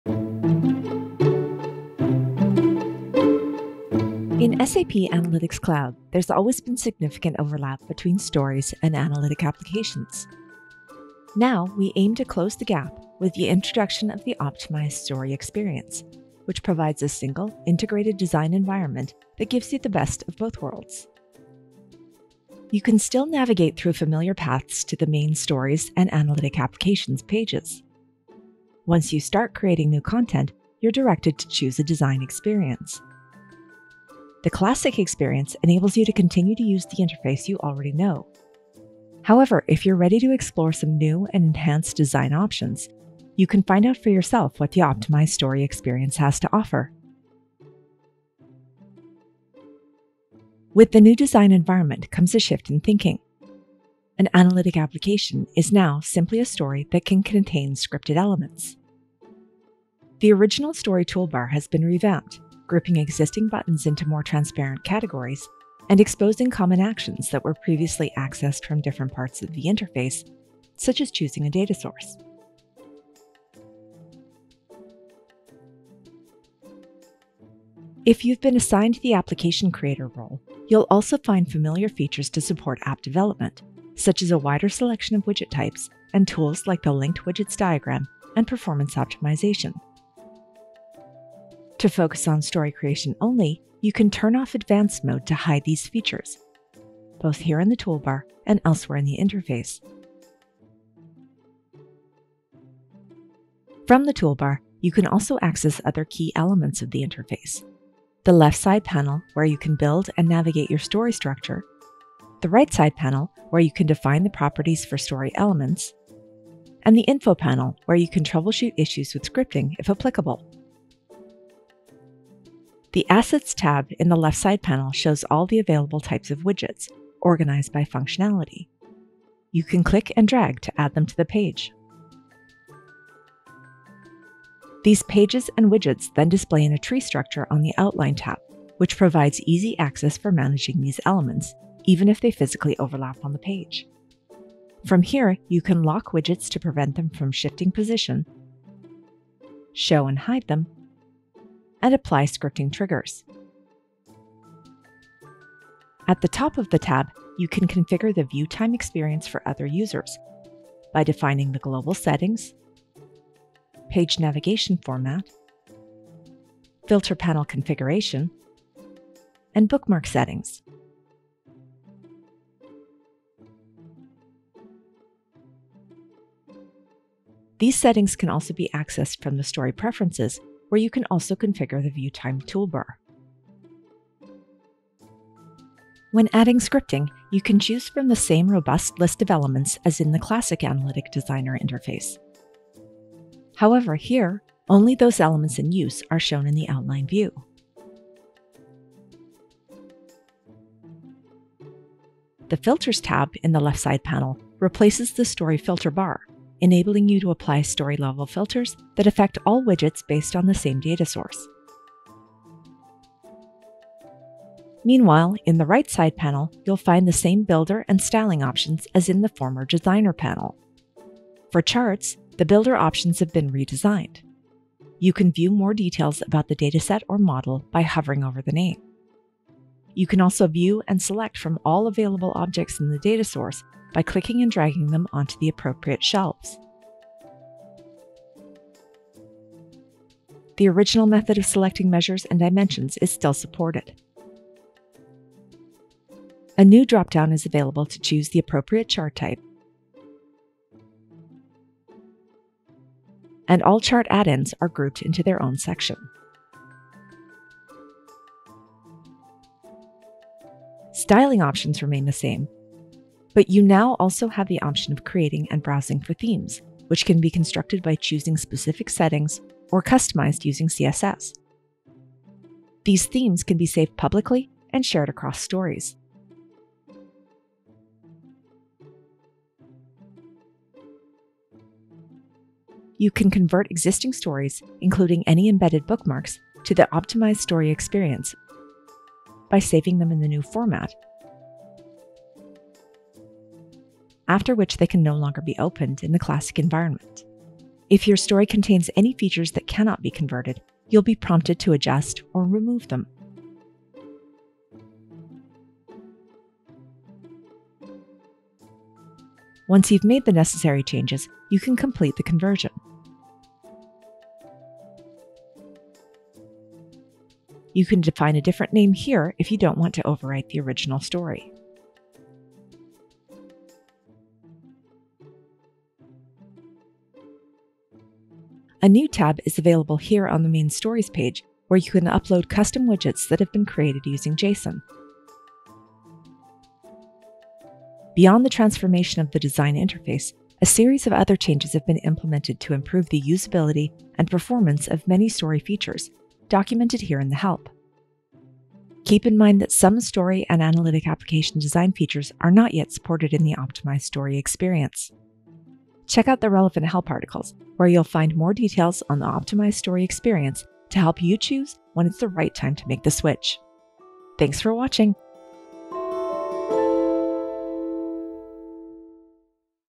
Introduction to the Optimized Story Experience: SAP Analytics Cloud
In this video, you will explore the features of the optimized design experience for stories and analytic applications in SAP Analytics Cloud.

Chapters:
0:00 Introduction
0:23 Choosing the optimized design experience
1:00 Toolbar and menu options
1:17 Application creator features
1:44 Left, right and info panels
2:03 Assets and Outline tabs
2:36 Navigation, filter, and bookmark settings
3:05 Adding scripting
3:22 Filters tab
3:33 Builder and styling options
4:19 Creating themes
4:41 Converting existing stories or applications
5:28 Custom widgets tab
5:37 About optimized design experience restrictions